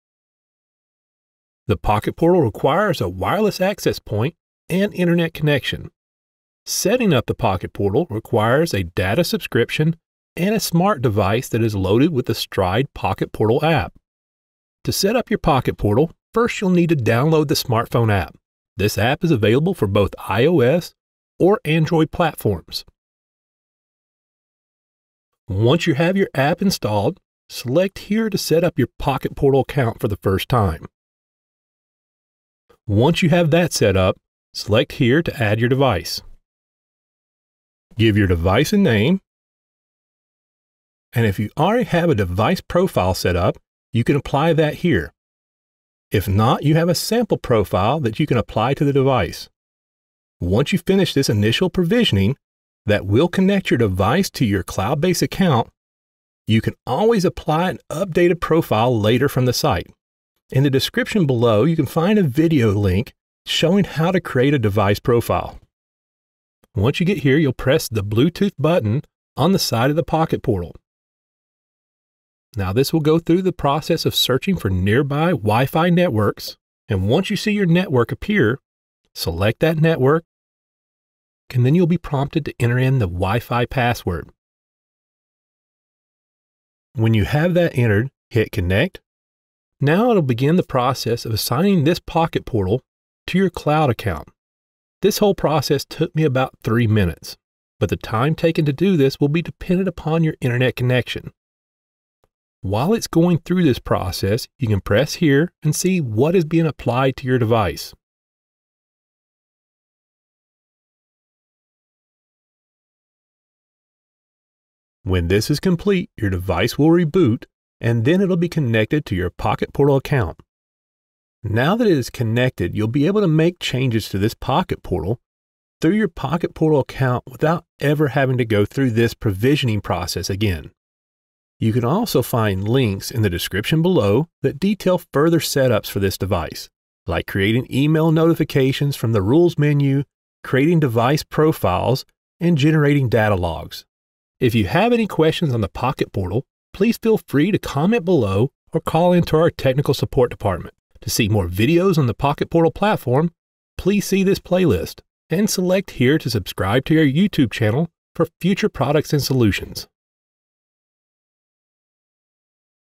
Stride Pocket Portal: Connect Your Device to the Account from AutomationDirect
- The STRIDE Pocket Portal is a simple, low-cost cloud data logger and storage solution that will gather and store data directly to the cloud from any device including analog sensors, actuators, and Modbus RTU controllers. The extremely compact size allows the Pocket Portal to fit anywhere and with WiFi capability, network connections are established without any extra network cabling. The STRIDE Pocket Portal is perfect for anyone needing a small DIY cloud-based data solution that won’t break the bank!

In this video, we show how quickly you can connect your wireless Pocket Portal data logger to your online account.

Other videos to help set up this product are:
Setup: Connect your device to your online account

Related AutomationDirect.com part numbers: SE-PB100,  SE-PP500K, SE-PP5M